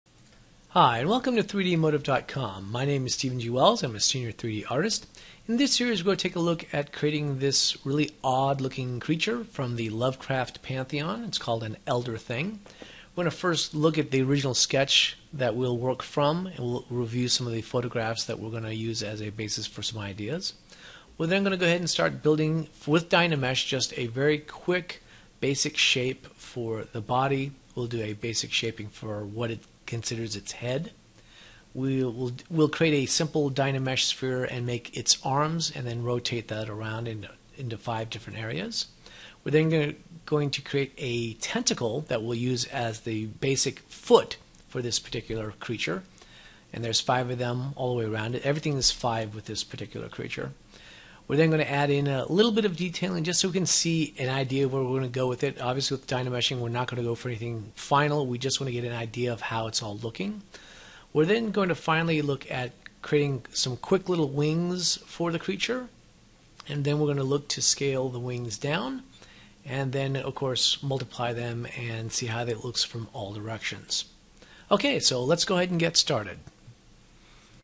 Monster Sculpts in ZBrush - Elder Thing - Pt. 1 - (3dm Promo)
In this series, we take on one of the very oddest creatures of the Cthulhu pantheon: the Elder Thing! We'll be using dynamesh for the entire alien entity but we're going to pull some "Frankensteining", that is, we're going to be creating different parts of the creature in different files to refine and finesse them before adding them to a singular IMM brush so that we can 'sew' together all the different elements into one cohesive, yet awesomely monstrous creature! We'll be running the gamut for detailing using brushes, alphas, layers and the IMM brush to end up with a very unique entity that would make Lovecraft proud!